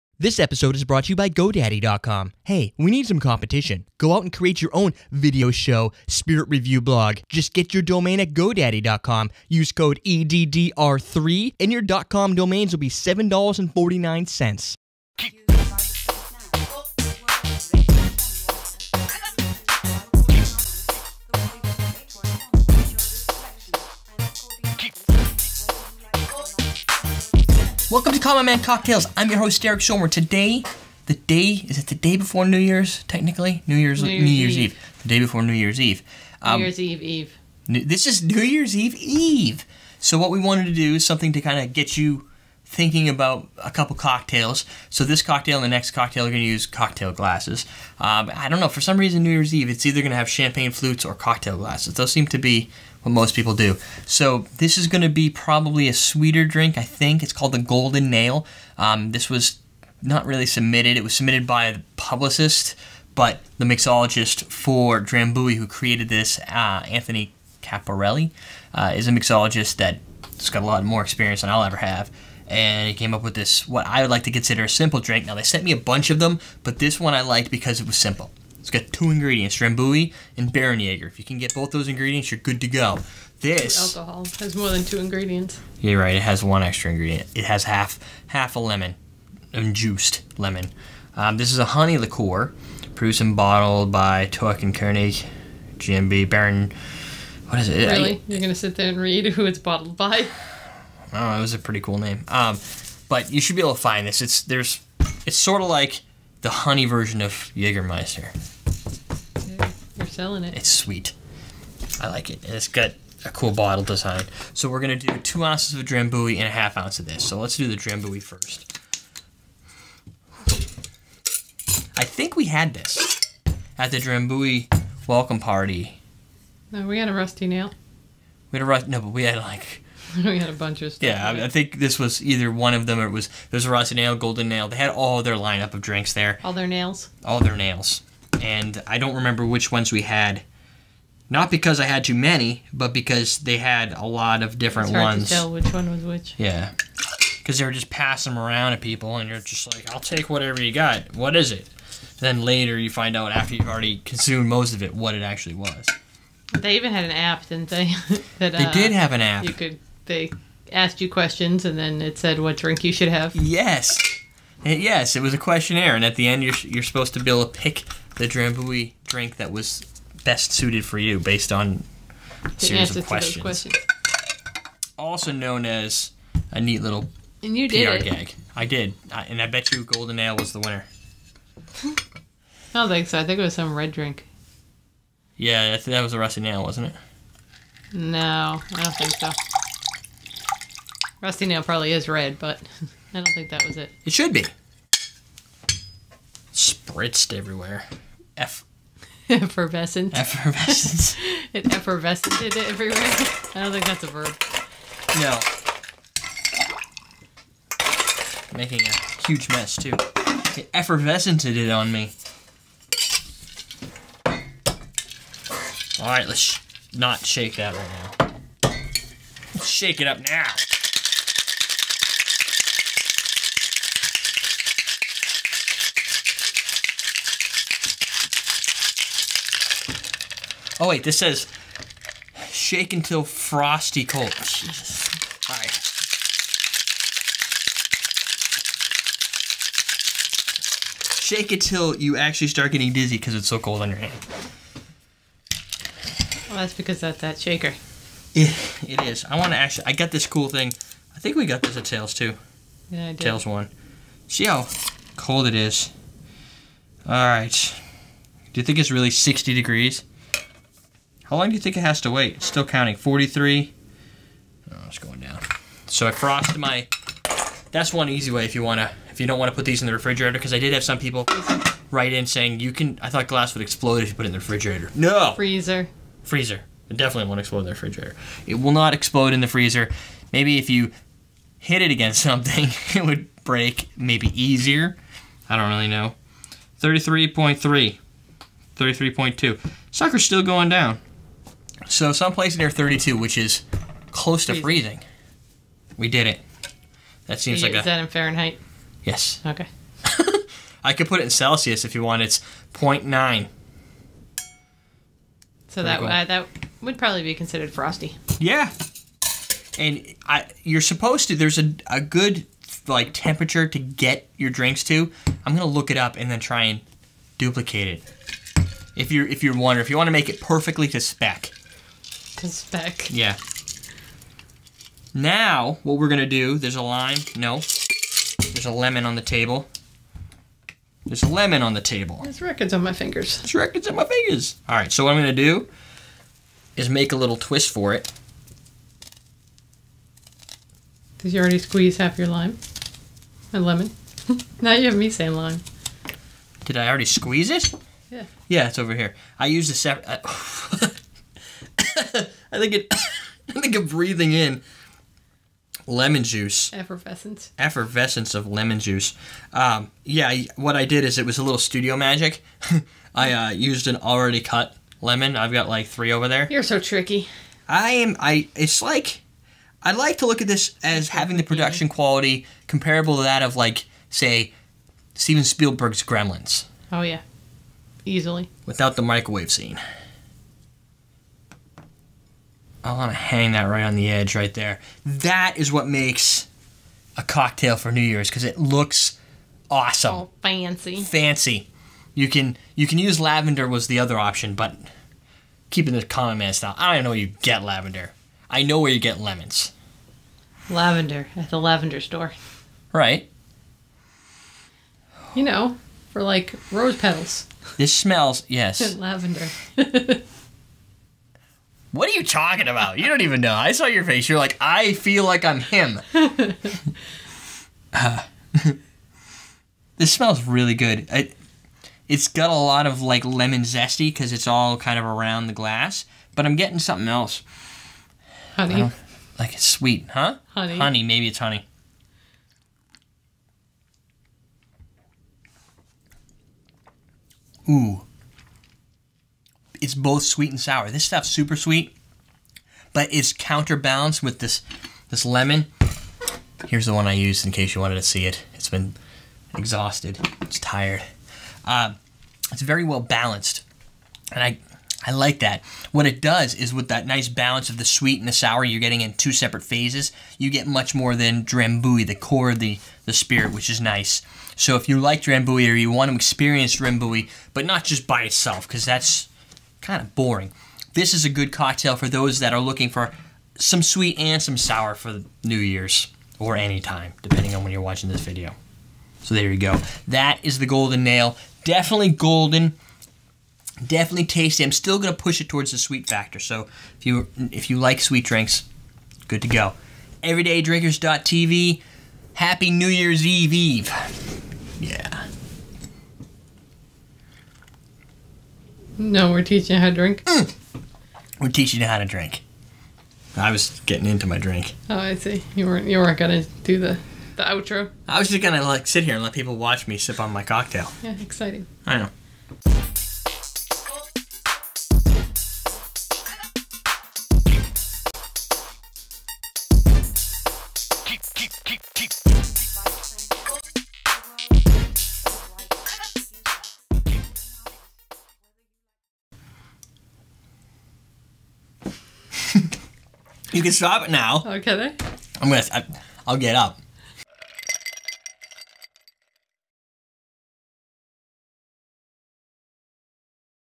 How To Make The Golden Nail
Zesty lemons with sweet honey, both sweet and sour at the same time. This recipe includes: 2 oz. Drambuie, 1/2 oz. Barenjager and the juice of 1/2 lemon.